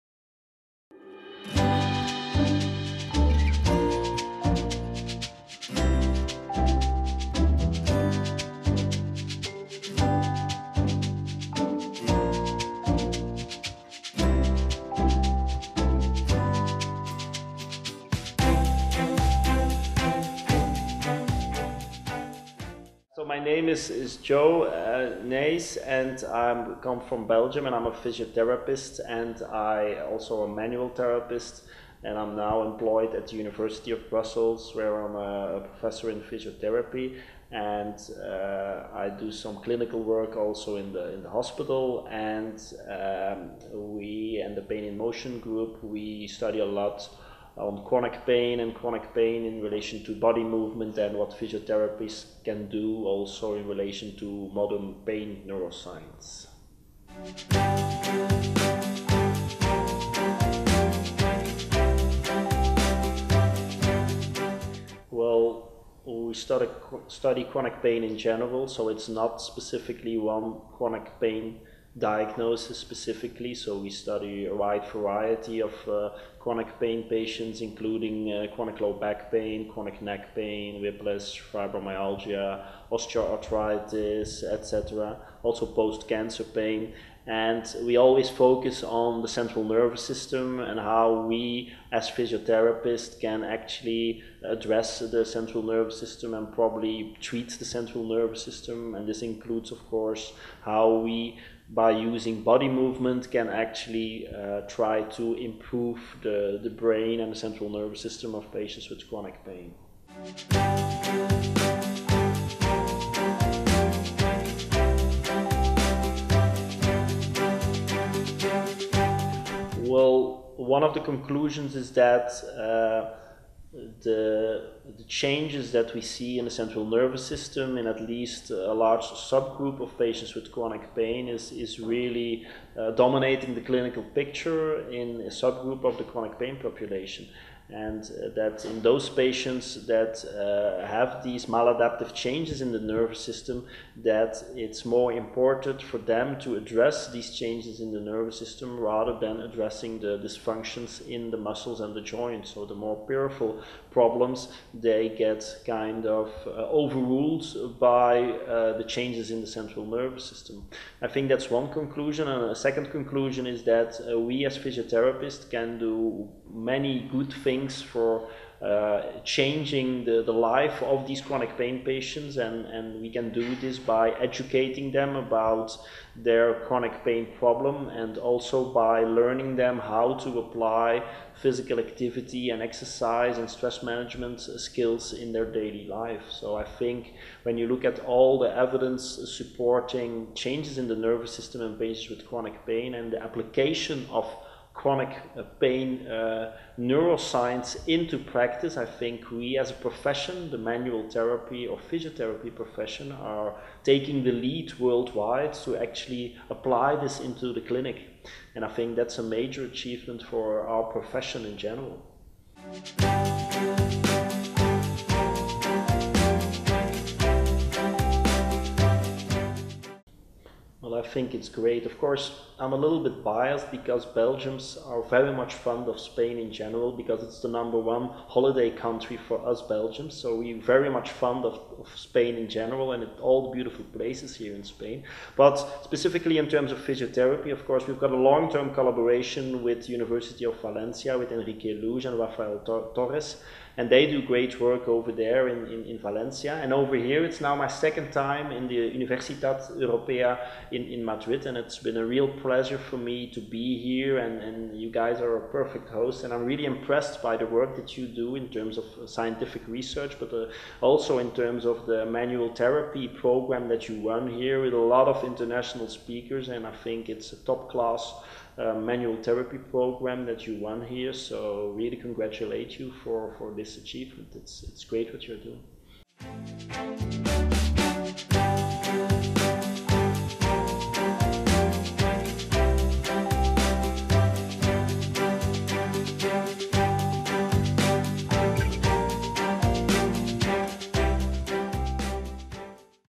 Jo Nijs en Master de Terapia Manual UE
Jo Nijs y miembros del grupo de investigación Pain & Motion hablan del dolor crónico y la sensibilización central en Master de Terapia Manual Ortopédica en el Tratamiento del Dolor de la Universidad Europea.